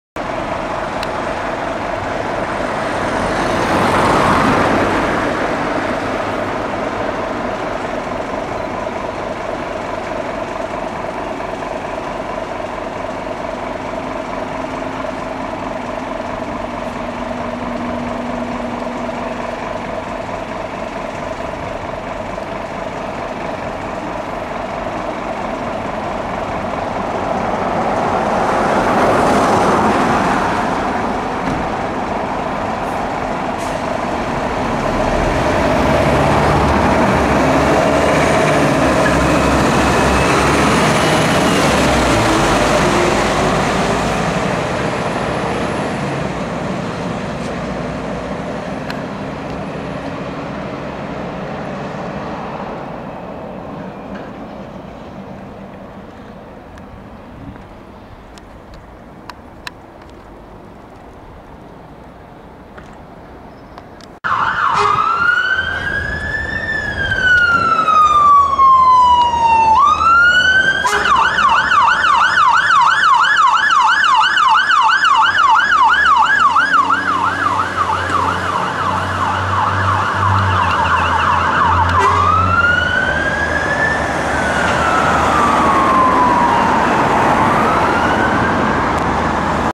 *NEW GENERATION SCANIA* Fire Engine Turnout/Reserve Appliance Responding
Something very special to bring you. Here we see a turnout from Dundee's Blackness Road Fire Station of the only New Generation Scania Pump to go on the run in Scotland as it currently stands. Followed by a Scania Reserve Pump to the same AFA.

This is the first proper video of an SFRS New Gen Scania Turning out!

Notice how the Reserve Pump has one LED grille light.

Note: All footage featured is my own work and therefore is copyrighted. Any and all attempts at infringement will result in action being taken against the offender.